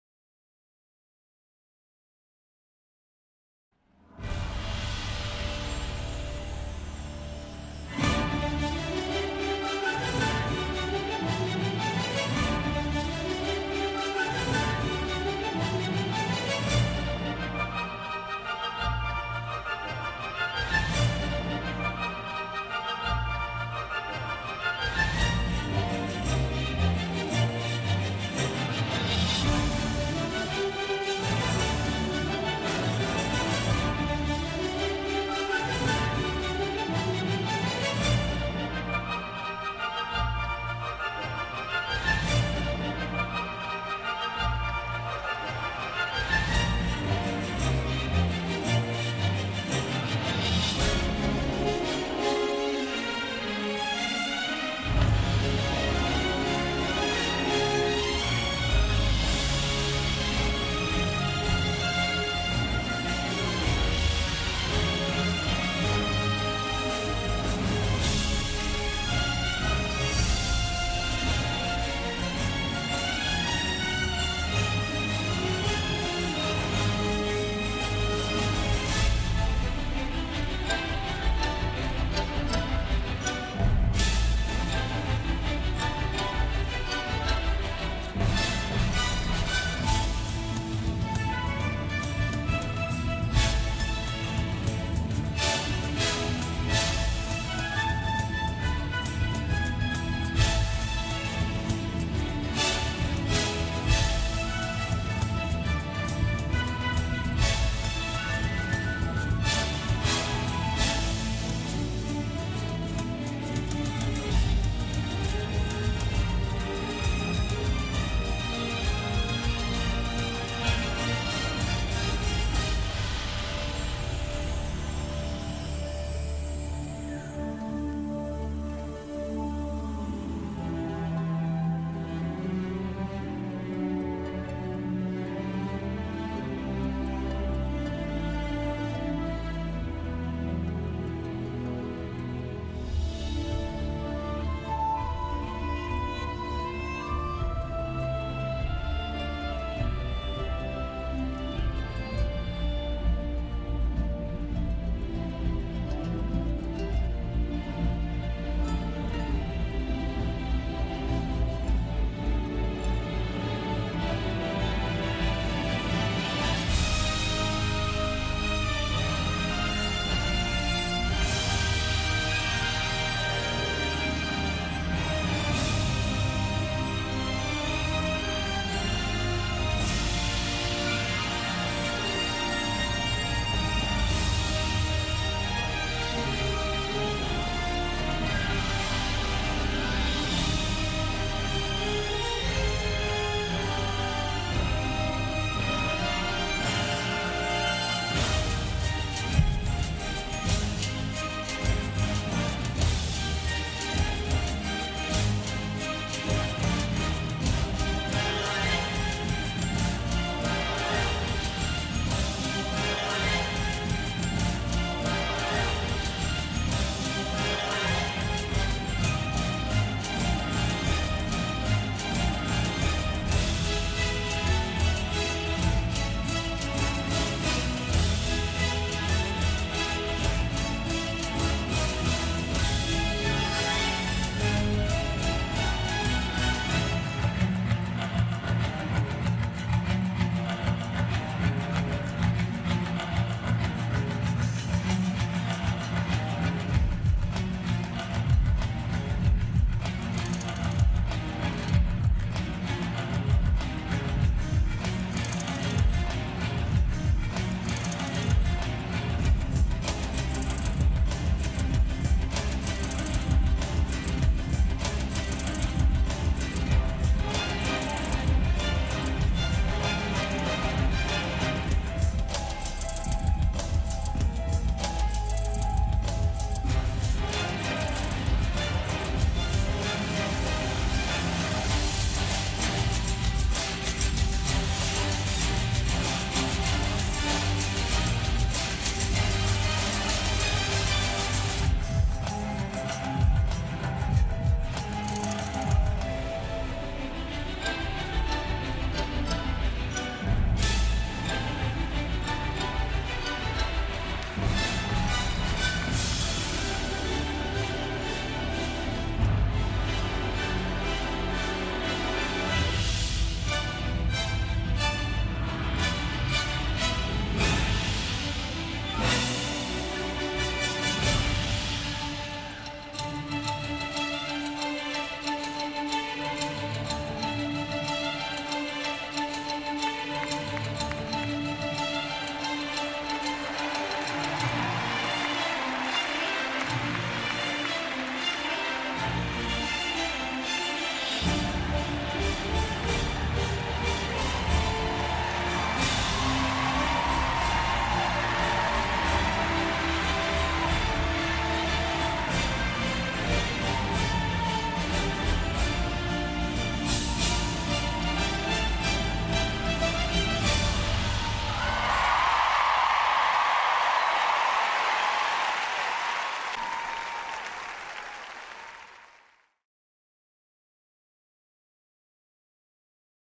Ashley Holzer & Pop Art,Las Vegas World Cup Final 2009
Ashley & Pop Art in their Kur--79.150% [5th place]